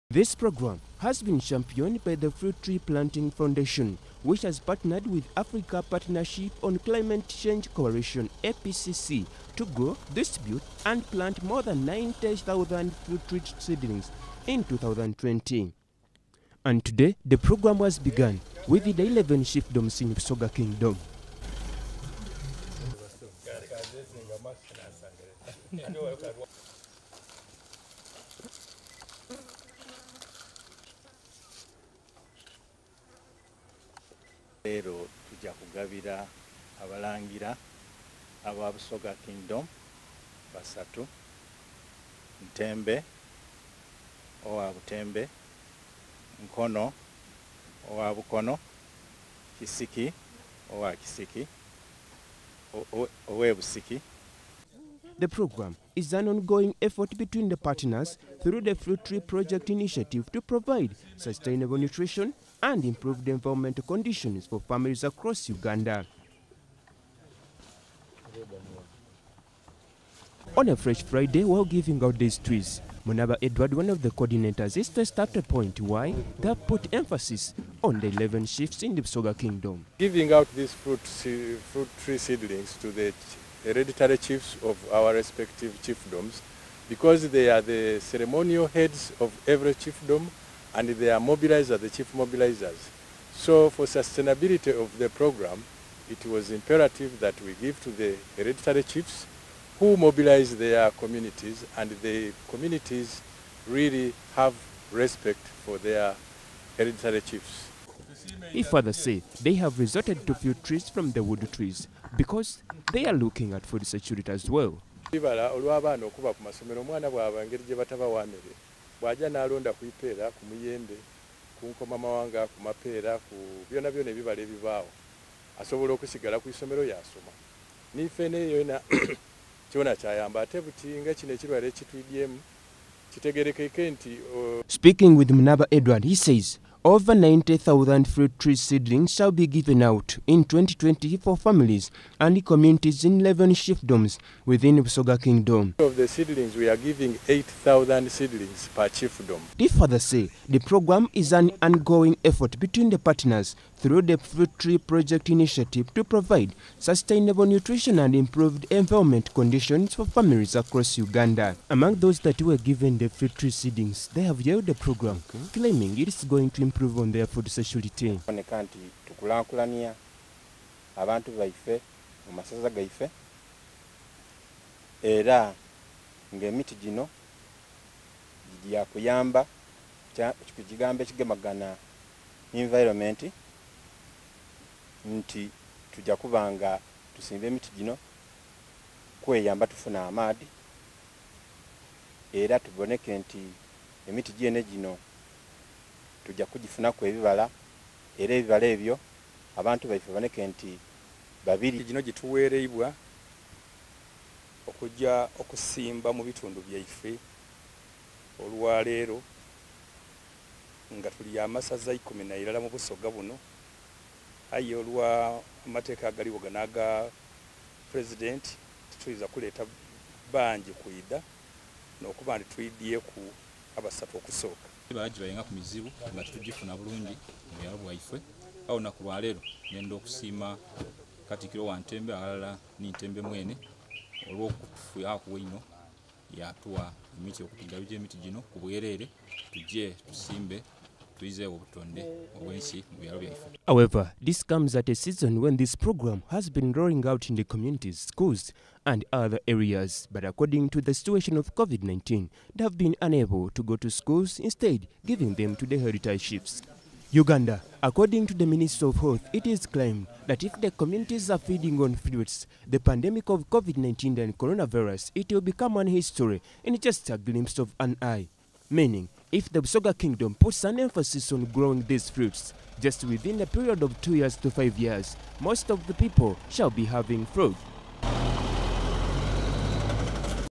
BUSOGA KINGDOM ON FOOD SECURITY
The Fruit Tree Planting Foundation(ftpf.org) has partnered with Africa Partnership on climatic Change Coalition (APCCC) to grow , distribute, and plant more than 90,000 fruit tree in 2020 for families and communities in 11 chiefdoms within the Busoga Kingdom . The program is ongoing effort between the partners through the Fruit Tree Project initiative to provide sustainable environmental conditions for families across Uganda